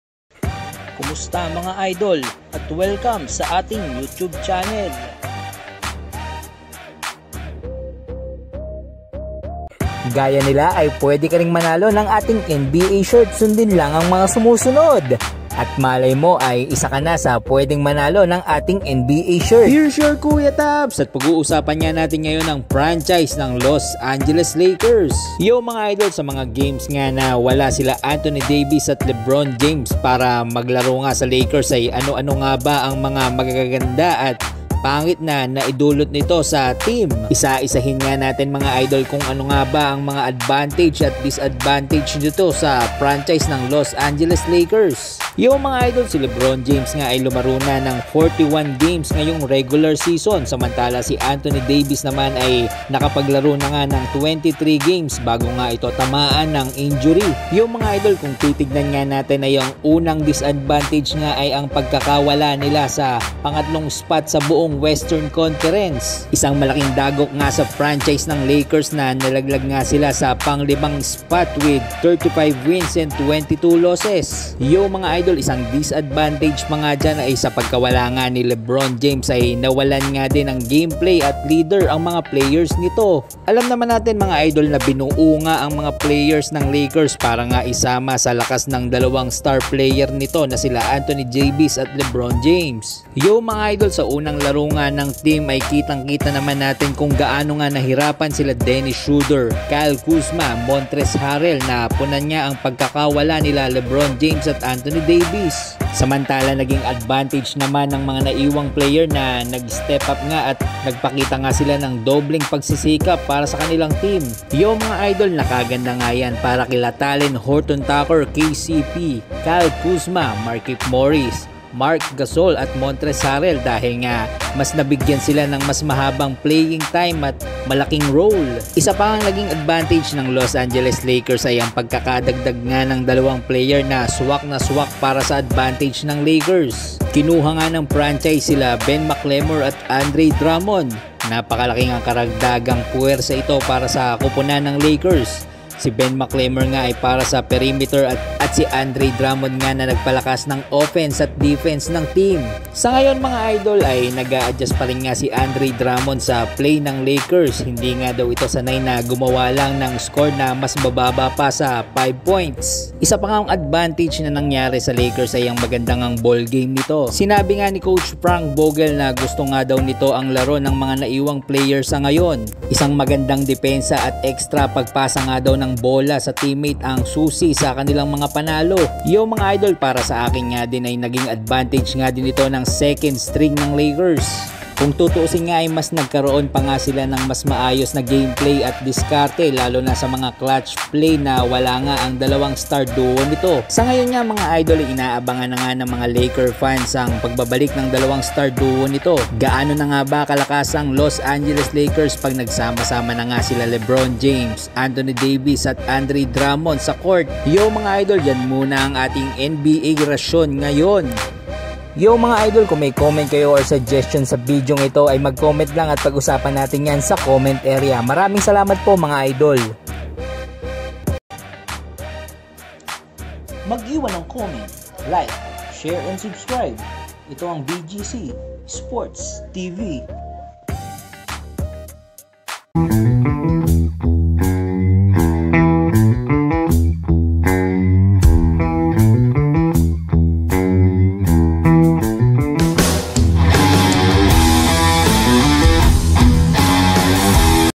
LAKERS coach Frank Vogel natuwa sa team, Andrei Drummond Naka Handa na!!!
yow mga idol isang masayang papuri nga ang nakuha ng team ng lakers mula sa kanilang coach

hindi nga biro ang mawalan ng dalawang superstar sa liga lalot patapos na nga ang regular season

mabuti nalang nga at sa bawat game ay may nag sstep up sa mga players ng LOS ANGELES LAKERS

ano ano nga ba ang advantage at disadvantage sa franchise ng lakers ng mawala nga ang dalawang star nito na sila anthony davis at lebron james

pag usapan naten yan mga idol sa comment area 🥰🥰💜💛